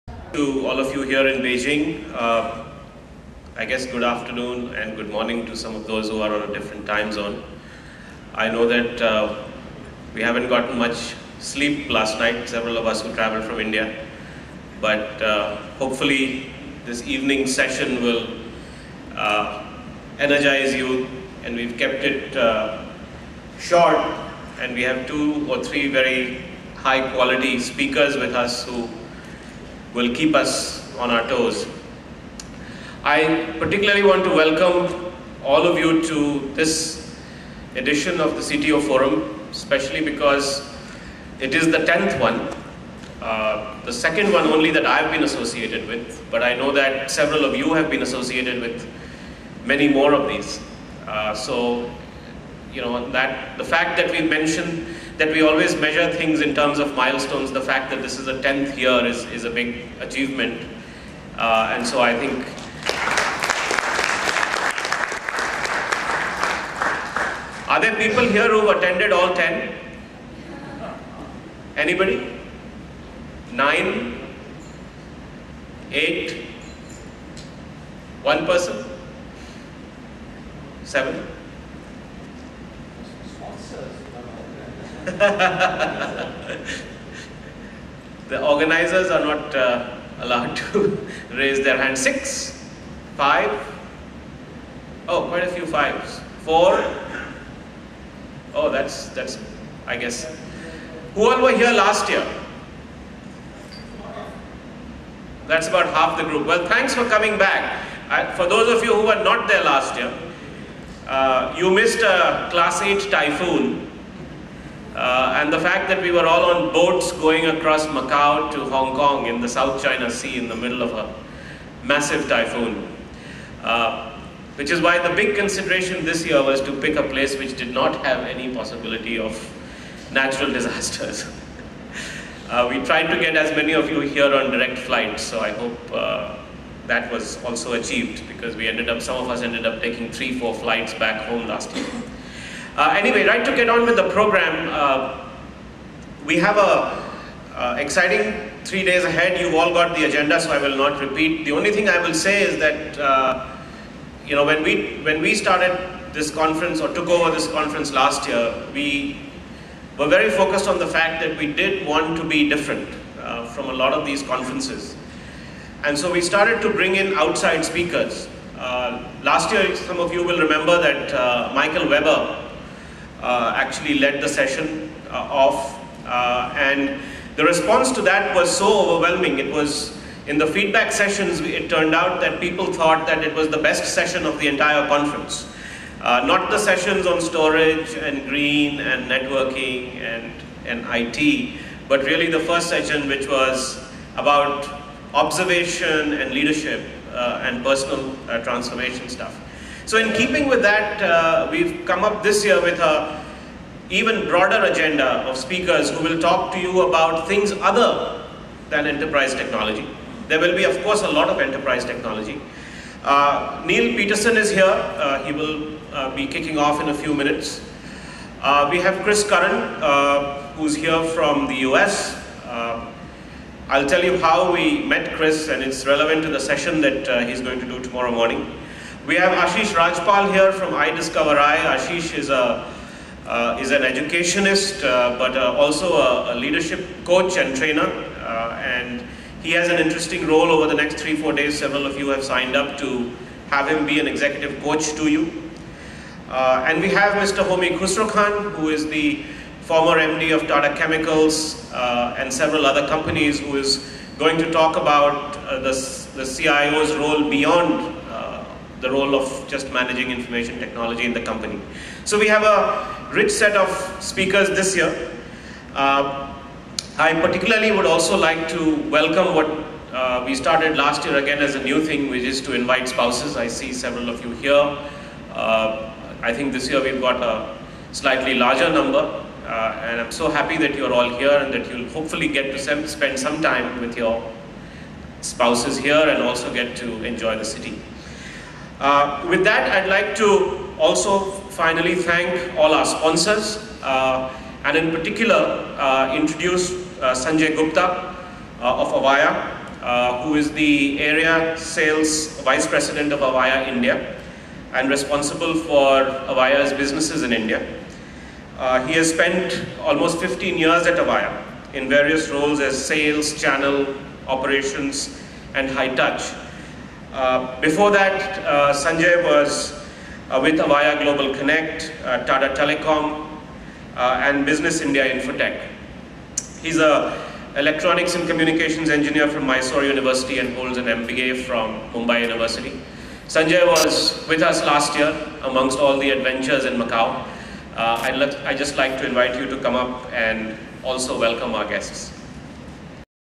WELCOME ADDRESS BY DR.PRAMATH RAJ SINHA
ANNUAL CONFERENCE OF CTO FORUM HELD IN BEIJING, CHINA FROM 23RD AUGUST TO 26TH AUGUST